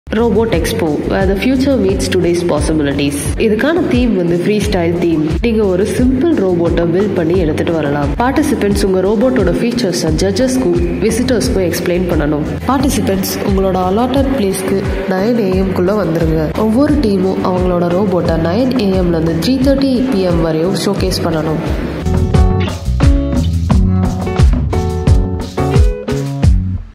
Robot Expo | Ekna2k22 Symposium | Anna University Regional Campus Coimbatore | Department of EEE
Description:
The robot expo where future meets today's possibilities and the participants from various department can showcase their robots in this expo and their robots will be scrutinized by the panel of judges and the robot which flabbergast the panel will get exciting prizes.

Rules and Regulations:
Team Participants should present before 9.00 am for registration and to get place allocation for their respective robots.
Team participating in this expo must showcase their robot in physical format
Every team should showcase their robot from 9.00 am to 3.30 p.m.
Participants should demonstrate their robots to the visitors and the panel.
Judges decision is the final.

Event Organizers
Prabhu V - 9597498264
Neena Nayak R K - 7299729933